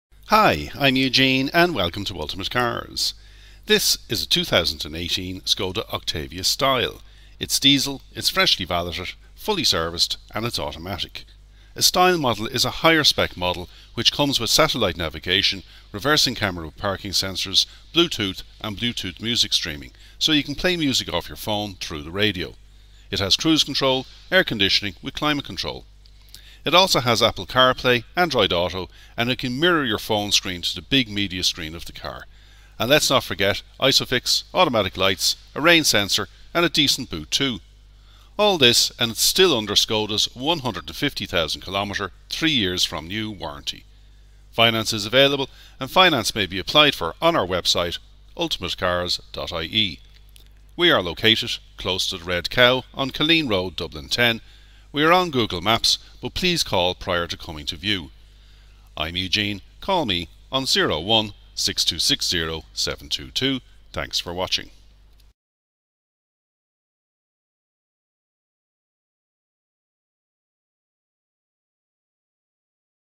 2018 Skoda Octavia Style
This is an immaculate Skoda Octavia diesel automatic.
It is still covered by manufacturers 3 years or 150,000 km warranty.
Extended warranties are available of up to an additional 2 years.

It has been fully serviced and valeted by us.

This one has Satellite Navigation, Reversing Camera with Parking Sensors, Bluetooth with Bluetooth music streaming, Climate Control, Air Conditioning, Automatic Lights, Rain Sensor and Cruise Control.